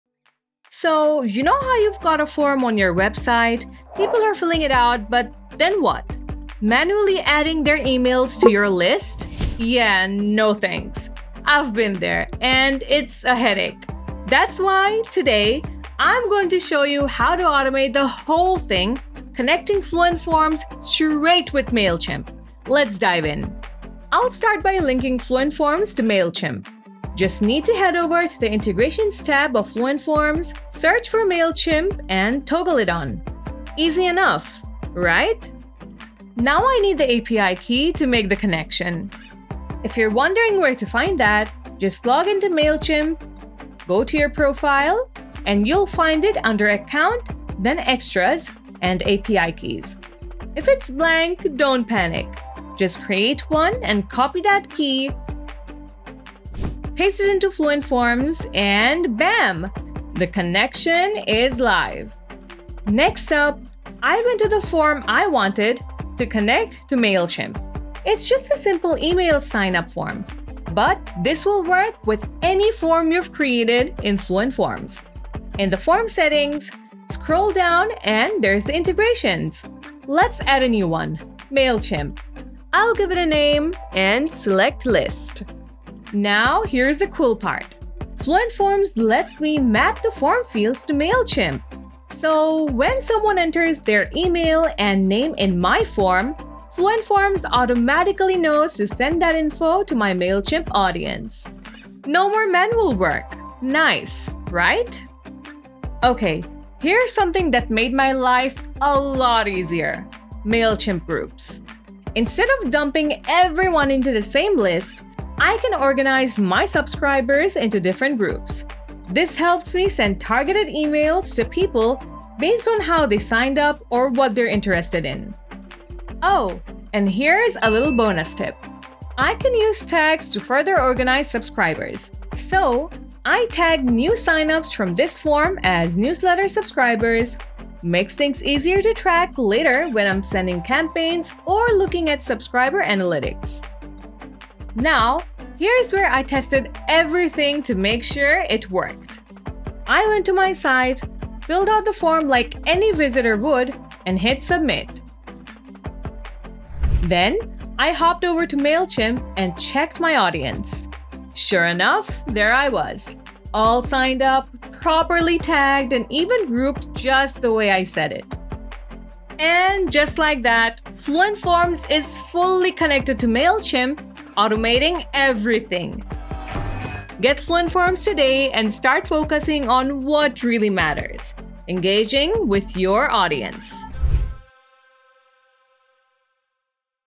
Connect Fluent Forms to Mailchimp
Simplify your workflow and boost your email marketing efforts by connecting Fluent Forms to Mailchimp. In this tutorial, you'll learn how to:
Effortlessly integrate Fluent Forms with Mailchimp
Automatically capture leads from your website forms
Grow your email list without lifting a finger
Segment your audience and send targeted emails
Say goodbye to manual data entry and hello to streamlined email marketing. Watch now to learn how to connect Fluent Forms to Mailchimp and take your online business to the next level.